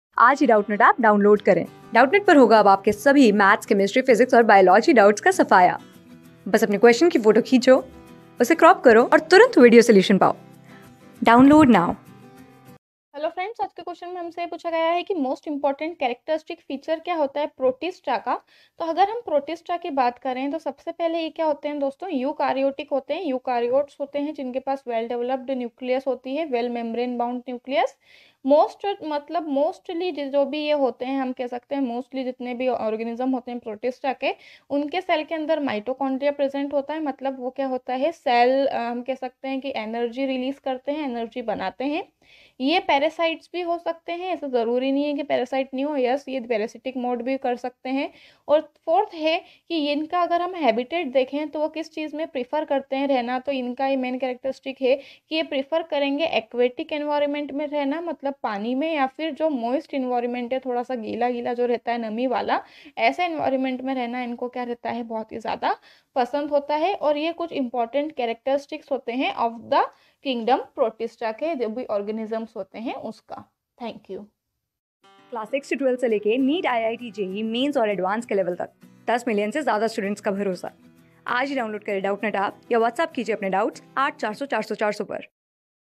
Write the most important characteristic feature of each of the following groups of organisms. Pr...
Write the most important characteristic feature of each of the following groups of organisms. Protista
Class: 9
Subject: BIOLOGY
Chapter: DIVERSITY IN LIVING WORLD - PLANTS
Board:FOUNDATION

👉 Have Any Query? Ask Us.